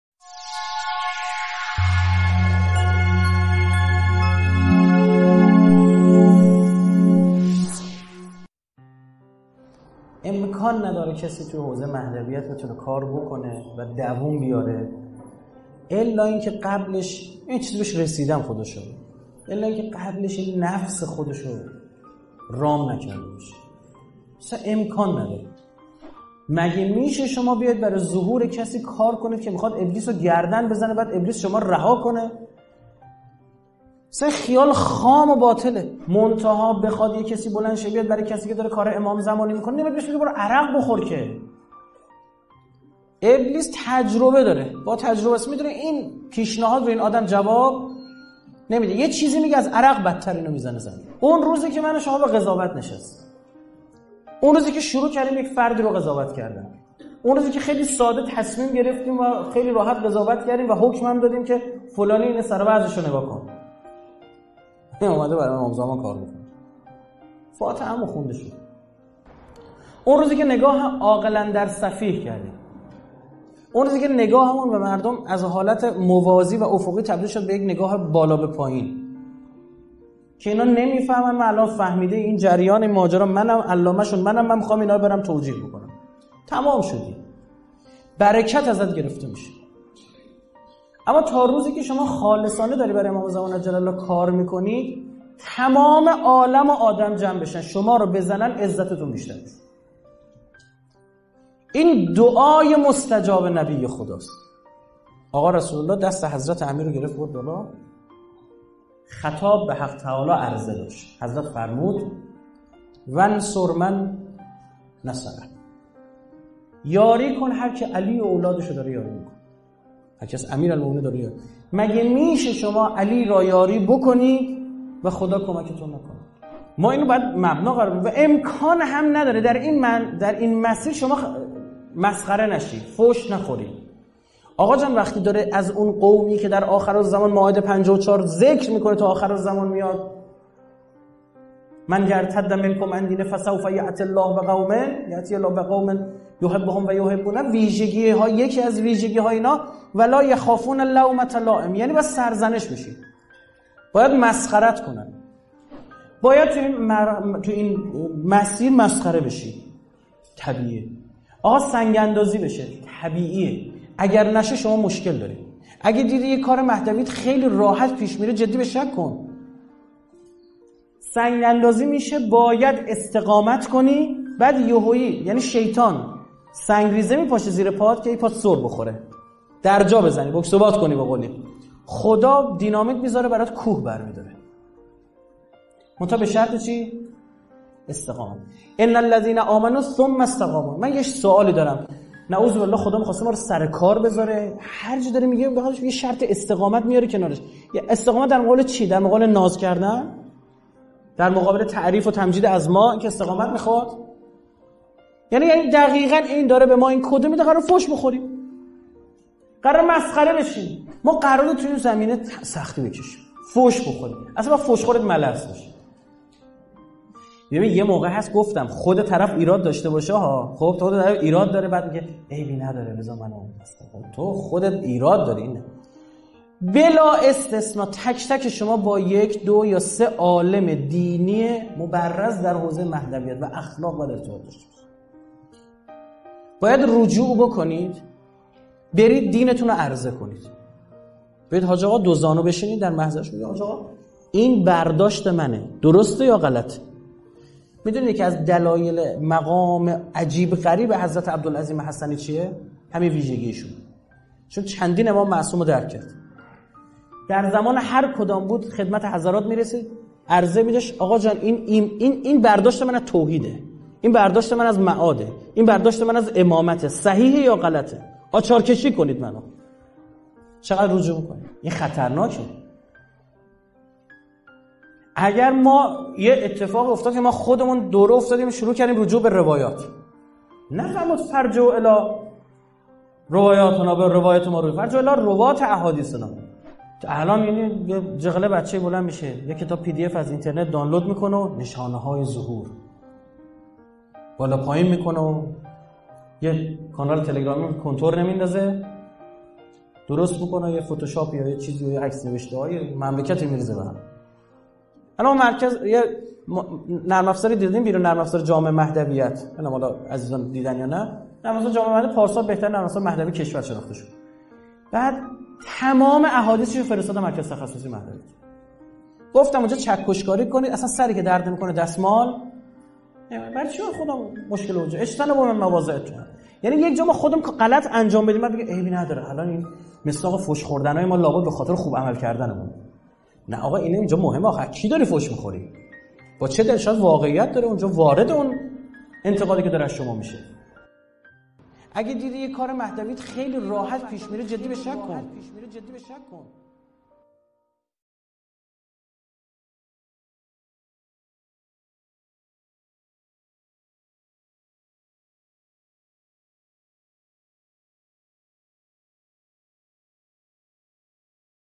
رائفی پور : کار مهدوی
کاربر ایتا:
🎬 کلیپ «کار مهدوی؛ قسمت اول»

👤 استاد

💢 امکان ندارد کسی در حوزهٔ مهدویت کار کند و دوام بیاورد الّا اینکه قبل از آن نفسش را رام نکرده باشد.